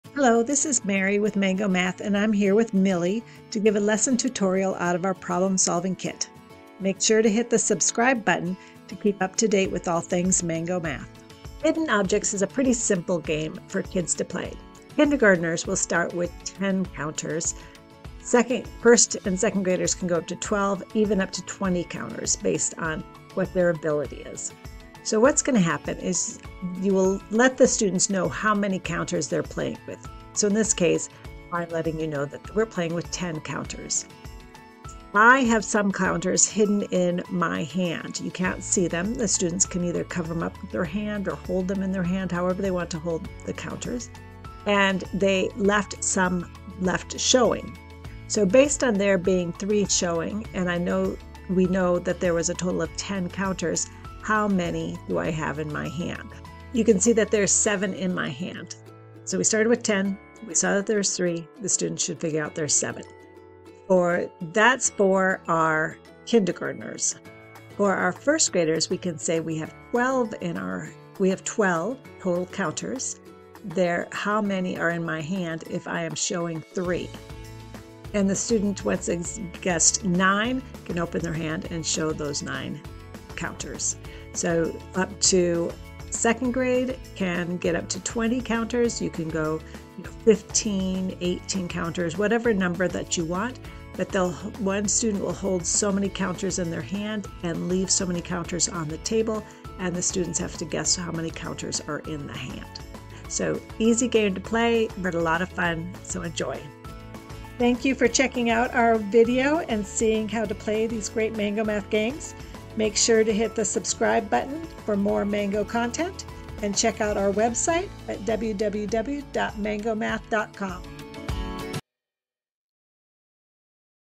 Hidden Objects (Problem Solving Math Lesson
Students play a fun game of hiding objects and students trying to determine how many objects are in their hand. Students know how  many total objects and how many are left on the table but must determine how many in hand.